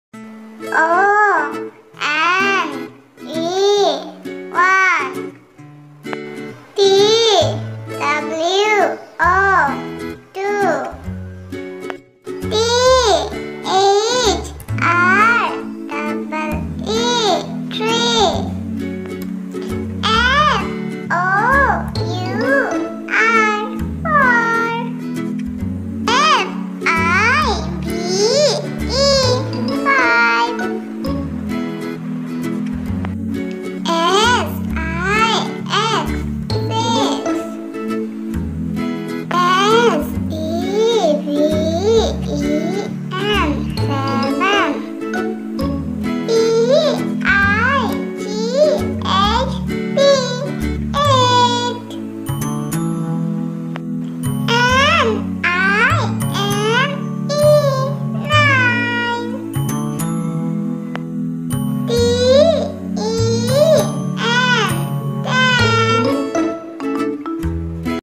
1234 Numbers song/number name/countings 1 to 10/
Hi everyone

This cartoon video will help kids to learn numbers easily. Visuals like cartoon are treat for kids.We create simple and fun loving video so kids can understand easily. We also use background music which makes it more interesting for kids.